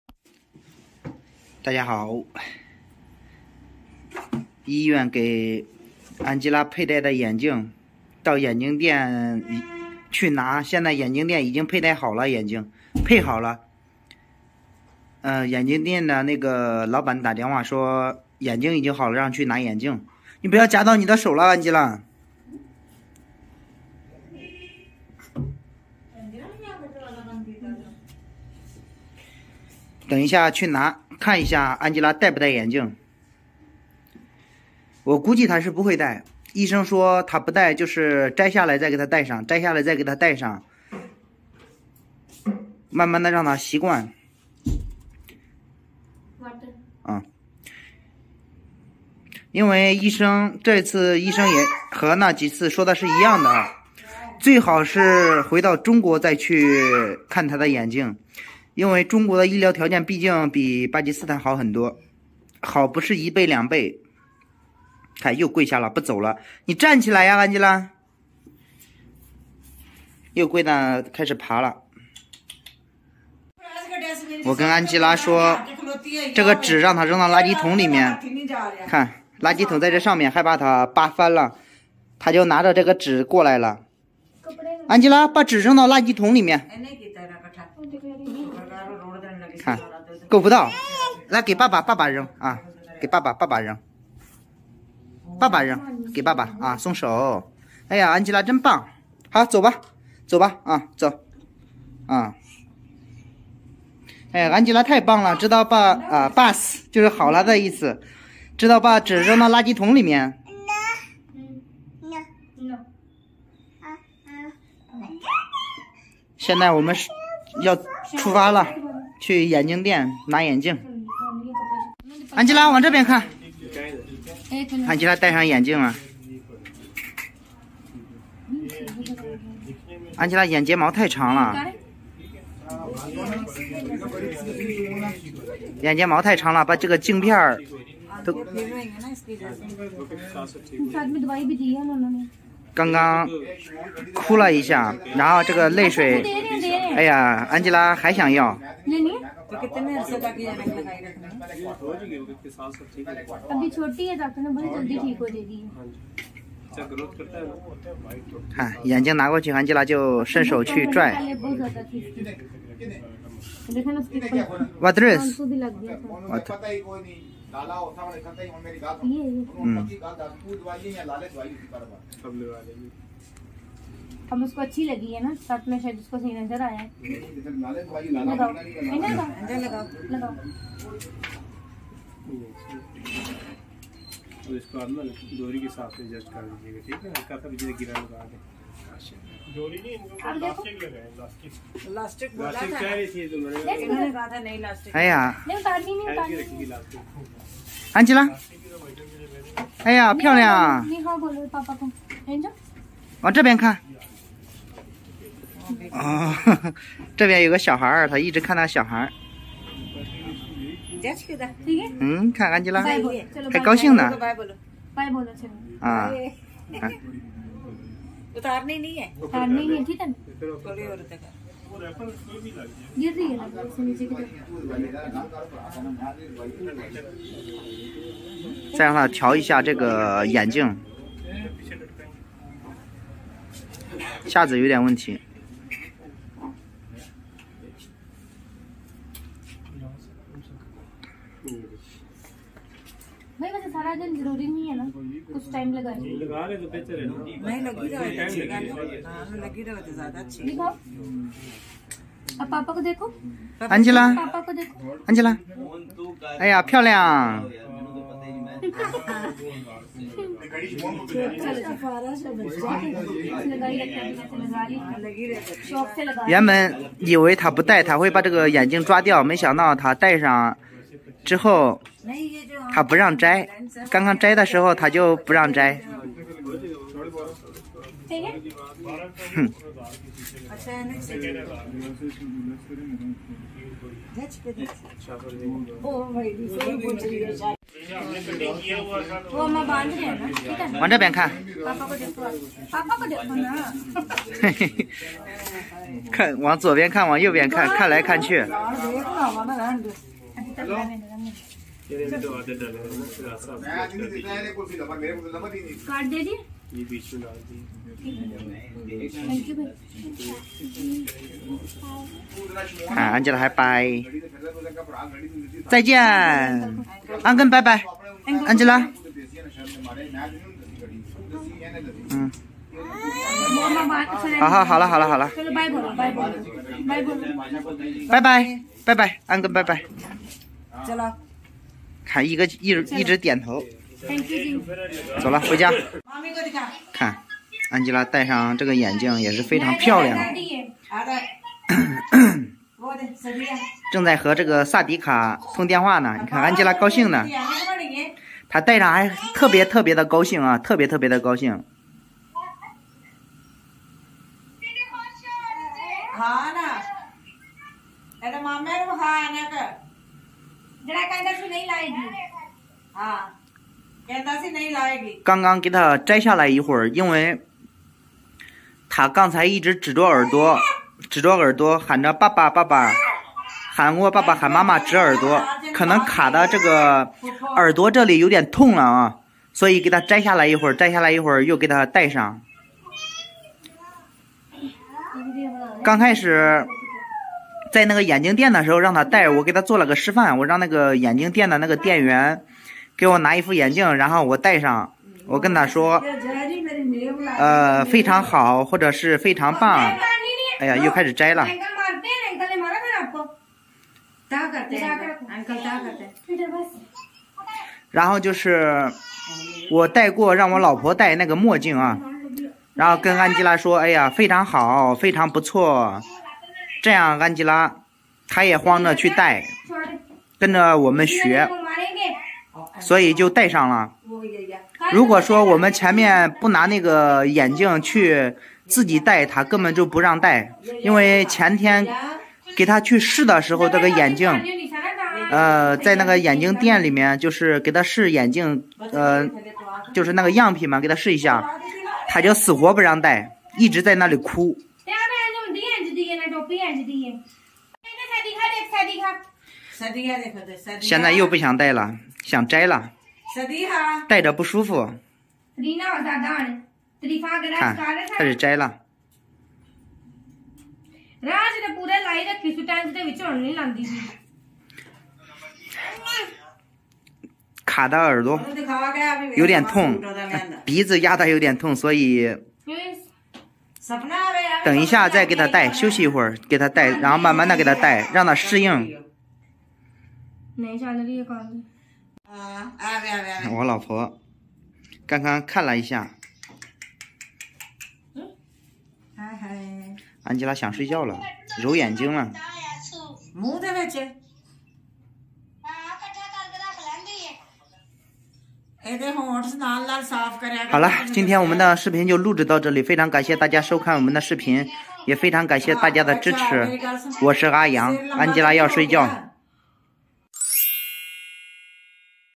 安吉拉第一次戴眼鏡，竟然這麽喜歡，戴上就不讓摘了
安吉拉第一次戴眼鏡，竟然這麽喜歡，戴上就不讓摘了【巴基斯坦沙塔吉】